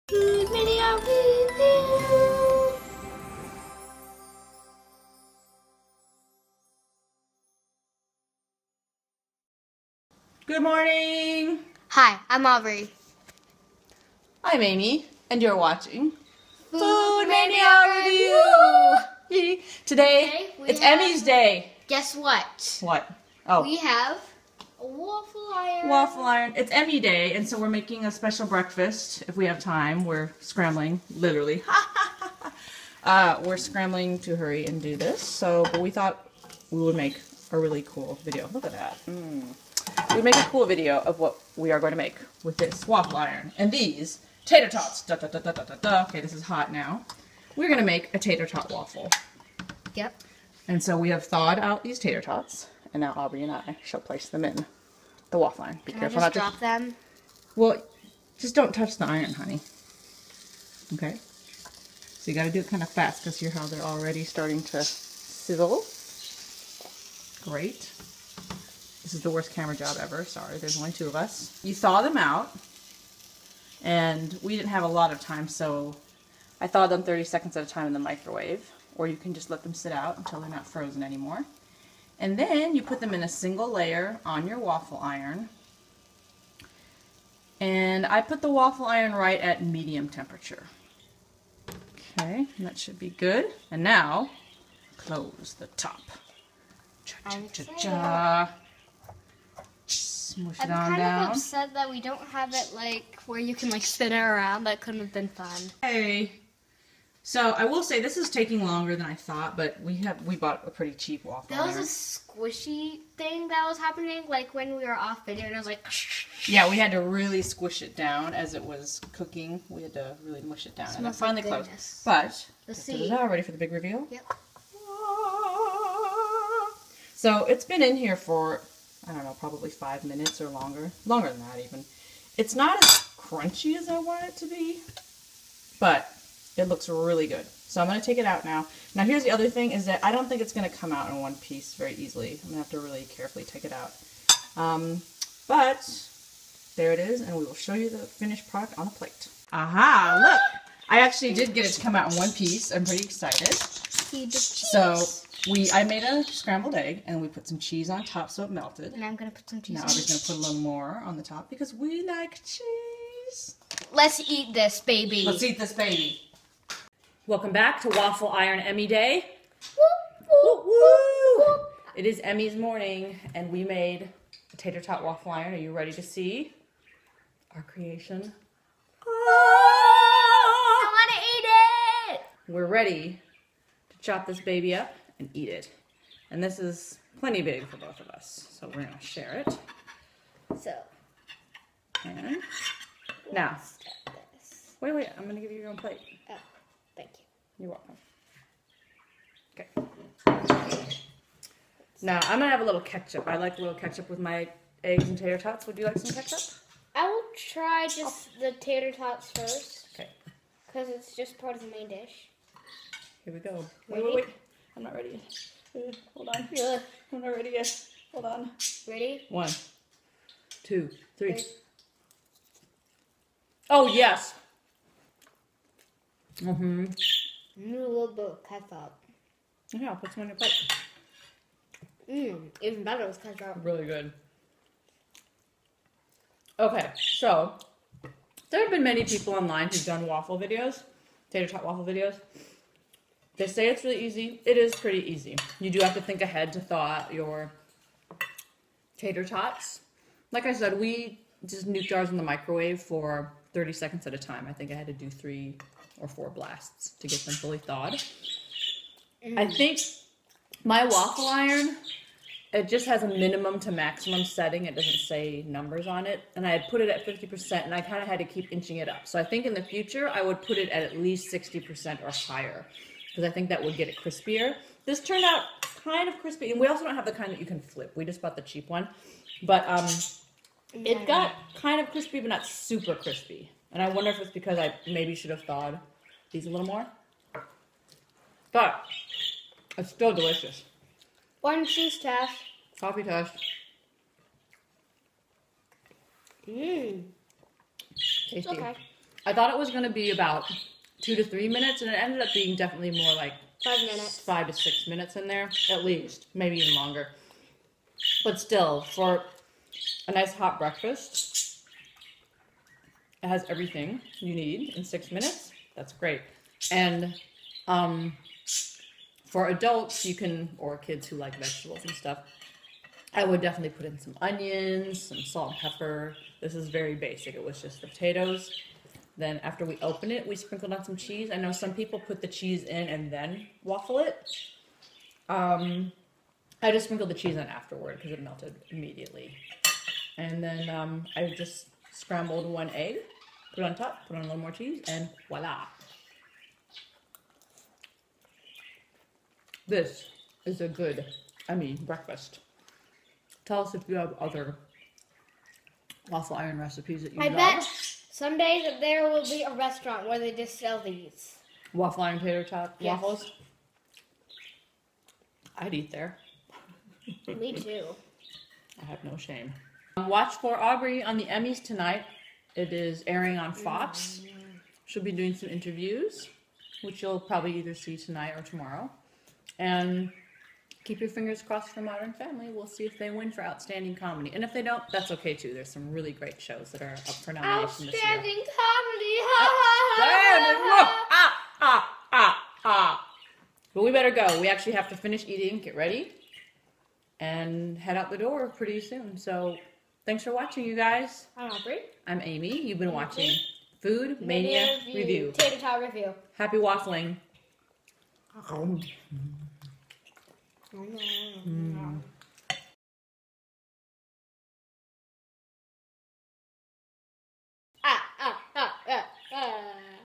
2015 Emmy Day Breakfast - TATER TOT WAFFLE
Twitter: @FoodMania Review  @AubreyLily  @TheAmyAnderson

Want us to try products from your town, state or country?!

Sorry for the late post today, but we wanted to do something special for Emmys day! We will be getting ready for the Emmys in just a few minutes and we wanted to make a great breakfast to keep us full for the long day. We got a waffle iron a few days ago and couldn't wait to try it out.

Here is our first attempt at waffle iron tater tots! See how it turned out. Watch for Aubrey at the Emmys on FOX tonight and pre-Emmy carpet coverage with ET and E!